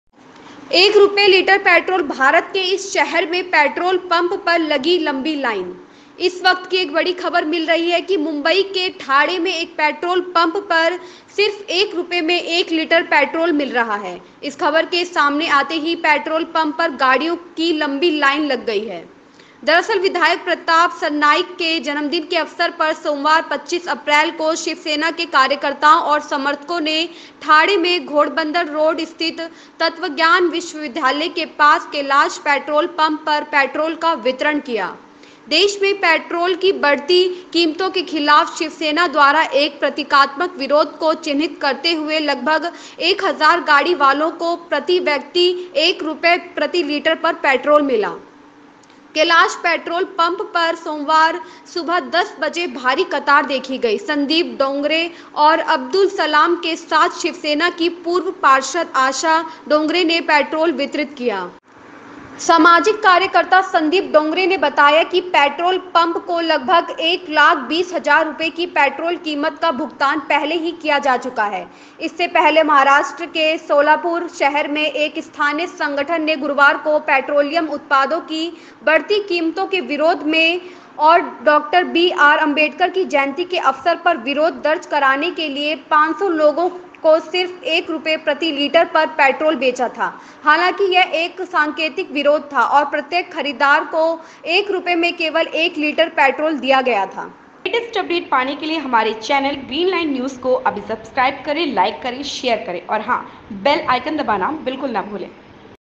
अभी अभी आई खबर कि भारत के इस शहर मिल रहा हैं एक रुपये लीटर पेट्रोल। Breaking news
More information---